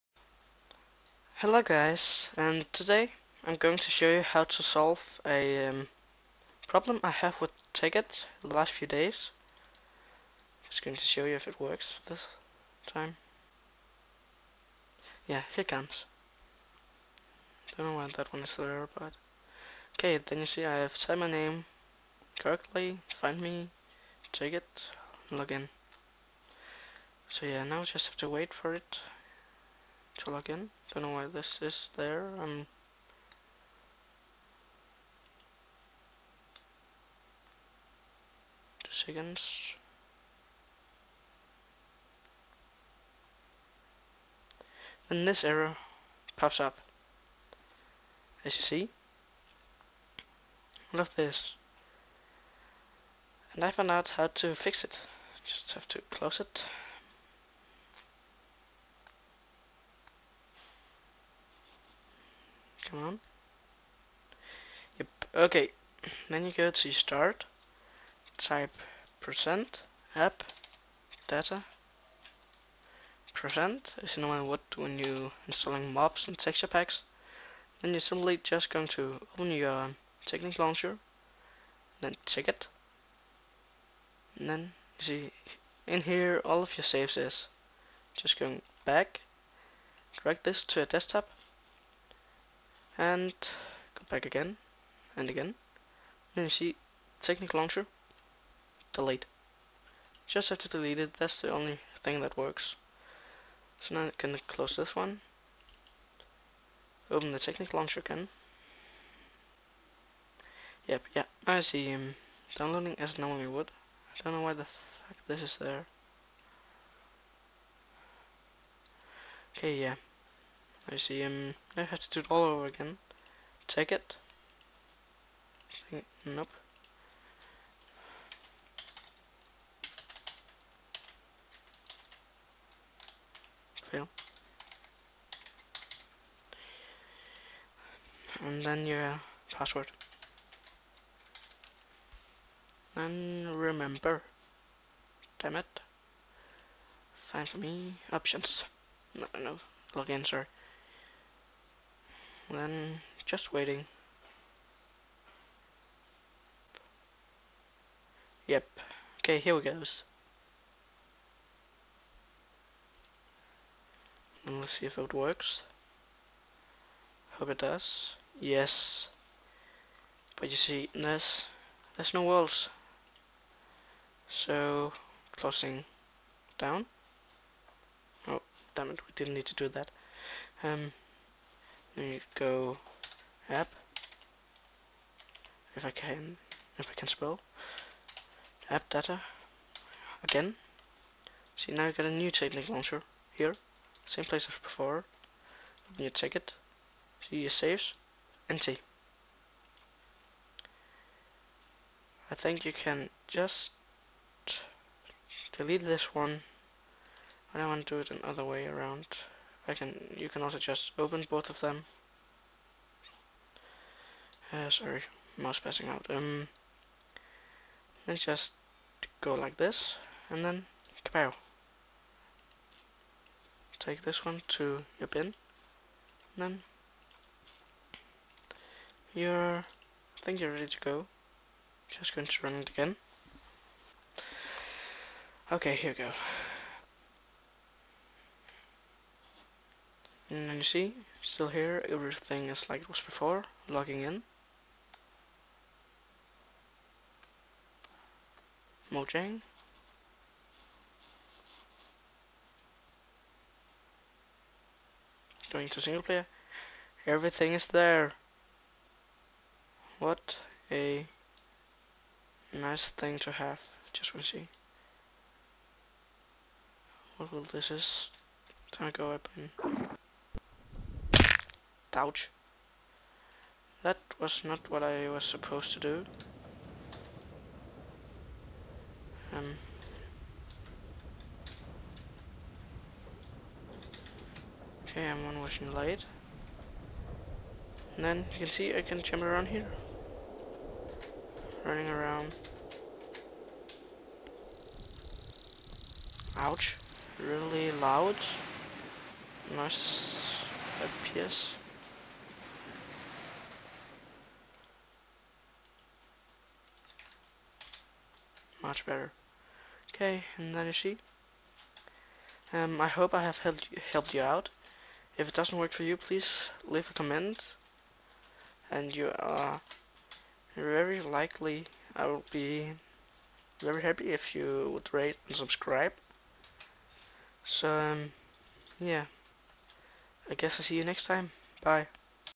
How To Solve Tekkit Error (e97e2f33)
Hey guys. Today I'm gonna show you how to fix the tekkit error: e97e2f33.

Plaese comment if it helped you out.

Plaese rate!